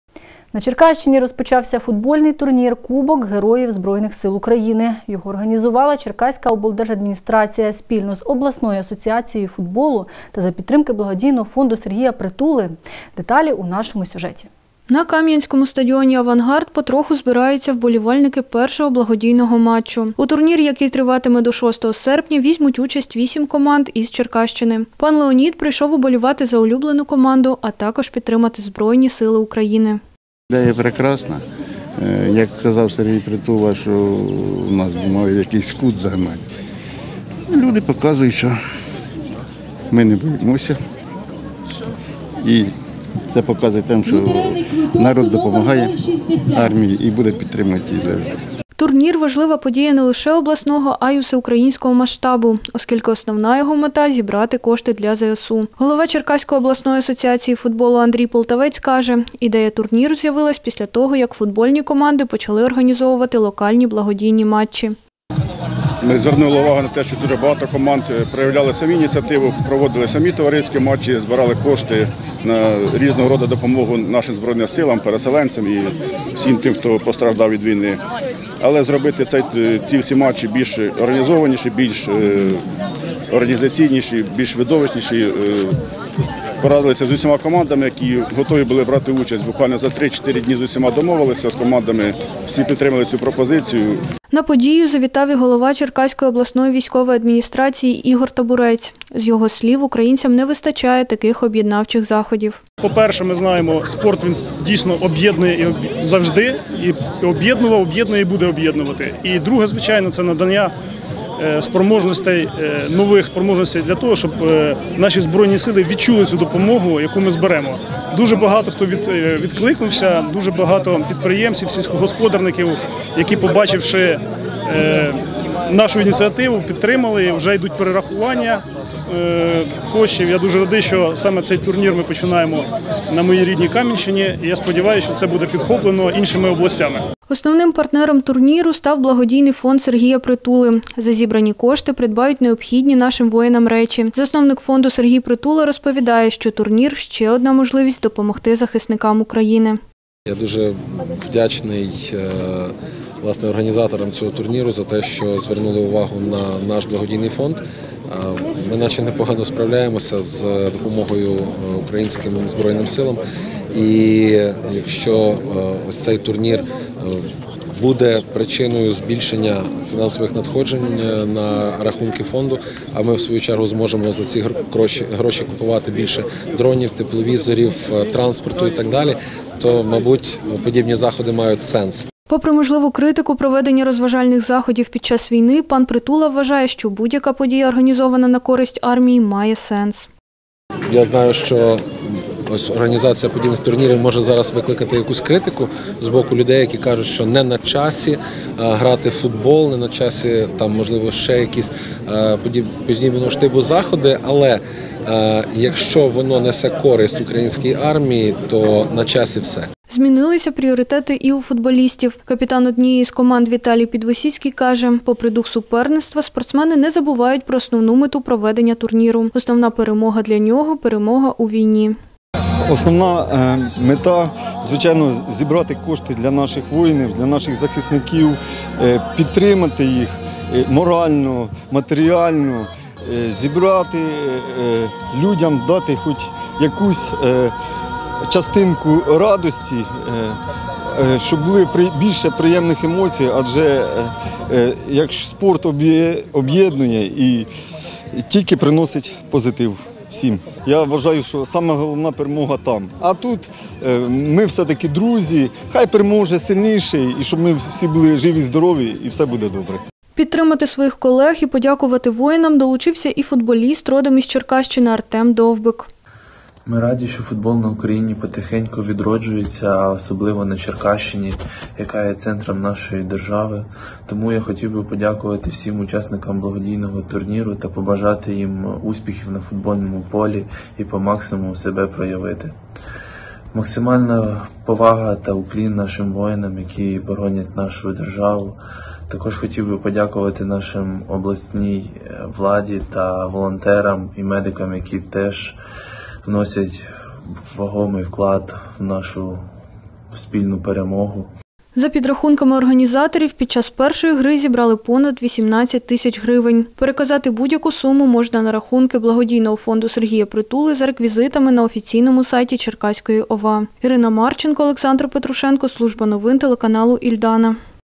На Черкащині розпочався футбольний турнір «Кубок Героїв ЗСУ»
Його організувала Черкаська облдержадміністрація спільно з Обласною асоціацією футболу та за підтримки «Благодійного фонду Сергія Притули».

Деталі у нашому сюжеті.